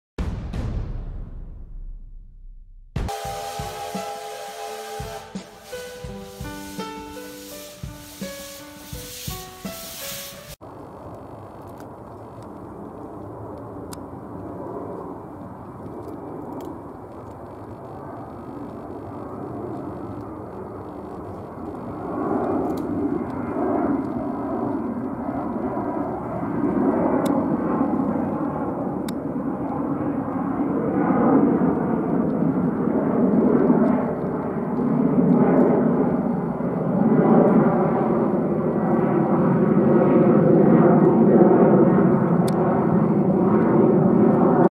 [90х] Самолёт Boeing 737-8LJ
Самолёт Boeing 717-8LJ (Приписан авиакомпания "Победа")
Рейс -
"Saint Petersburg - Volgograd".

Санкт-Петербург,Россия.
28.X.2020